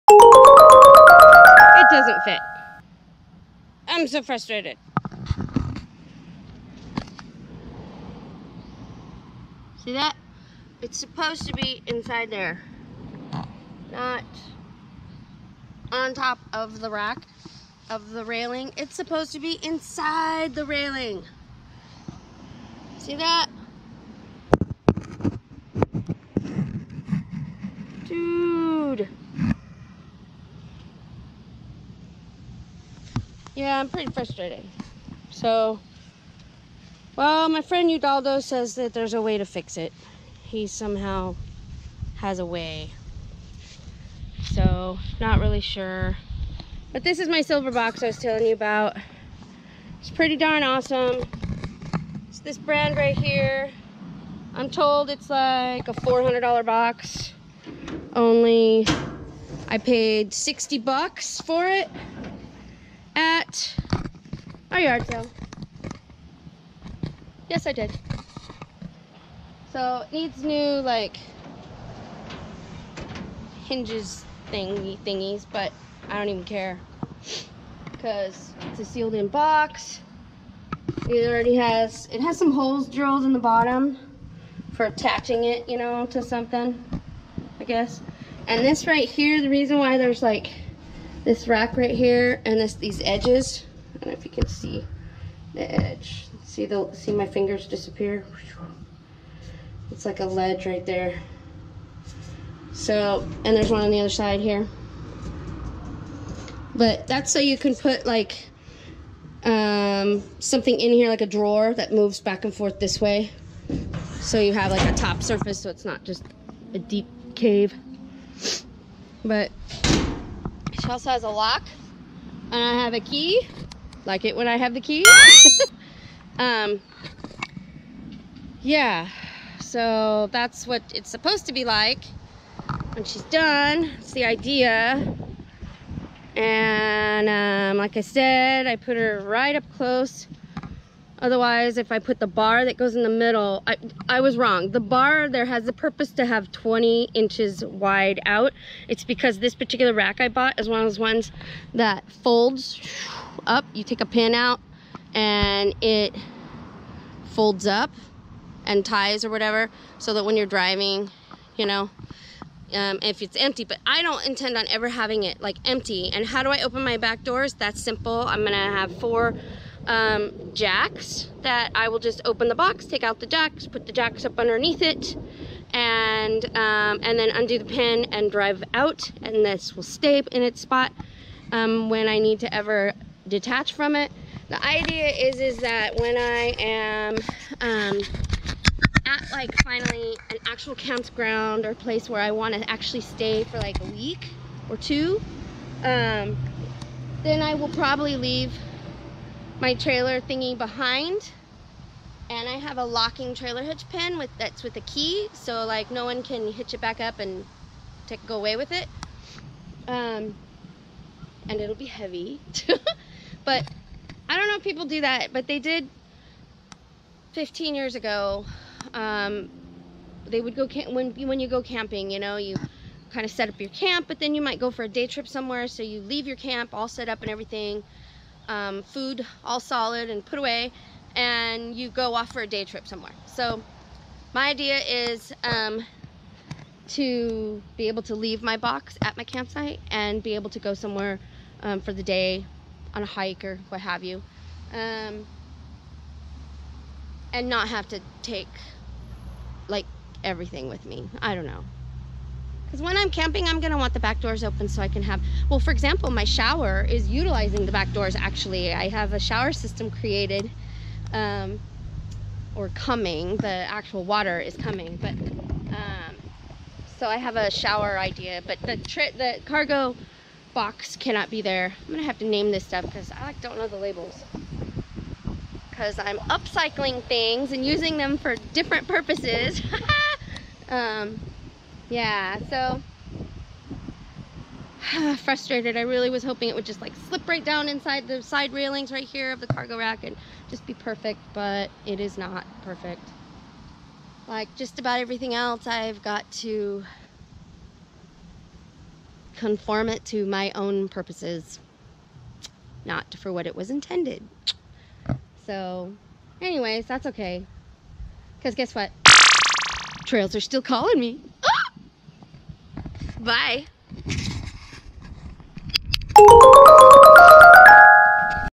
ItDoesntFit
I bought this hitch cargo rack specifically because the description said that the INSIDE dimensions were the same as what I measured  the outside dimensions of the metal box is, thinking it would just slip right into the rack safely. I was wrong. Rather the rack dimensions are wrong because they curve IN at the 4 corners. AUGH! Help! lol